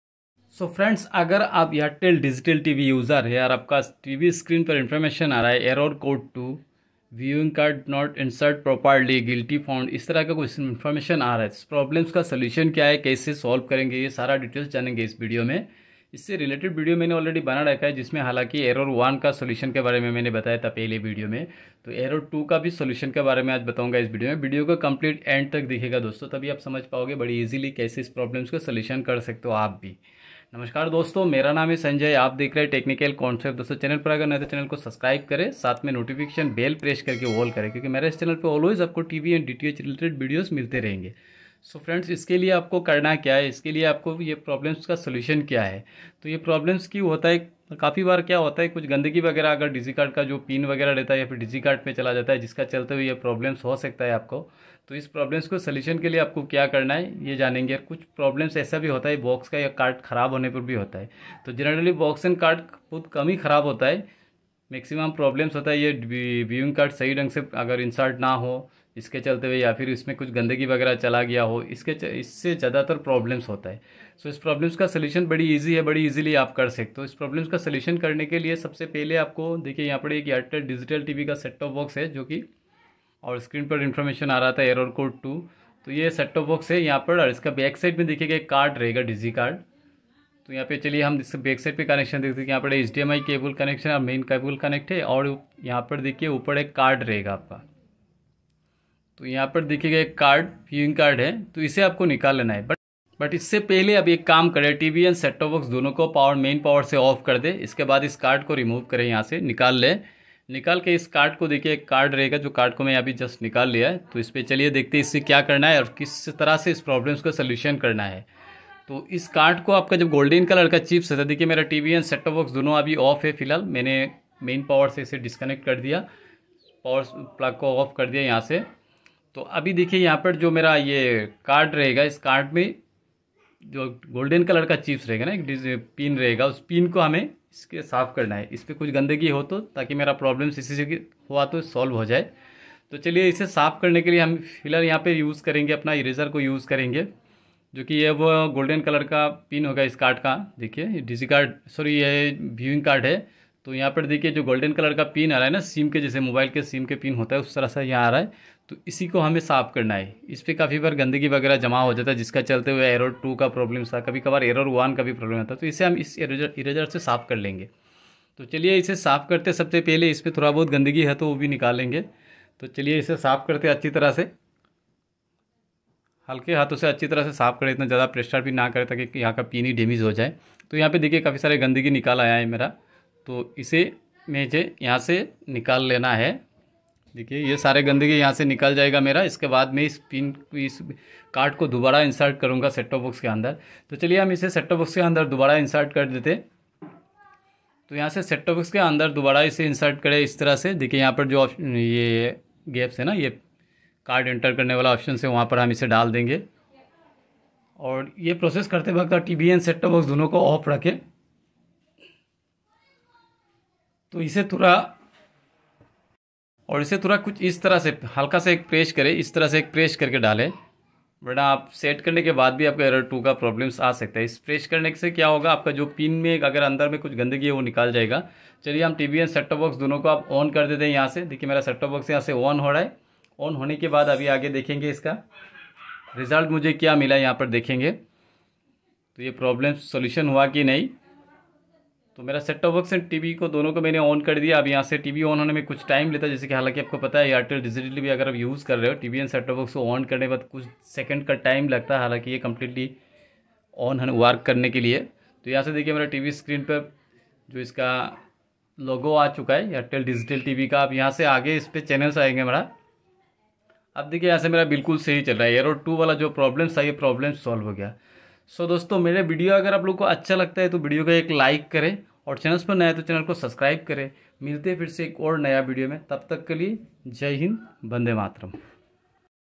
Viewing card is not inserted properly airtel dth // Airtel dth error code 2 solution
viewing card is not inserted properly airtel dth
Hi friends I am Sanjay Technical concept 1412.In this video I have shared with you guys that?
1) viewing card is not inserted properly airtel dth
2) Airtel dth error code 2 solution
3) Airtel dth viewing card problem
4) Airtel dth viewing setting

Disclamer-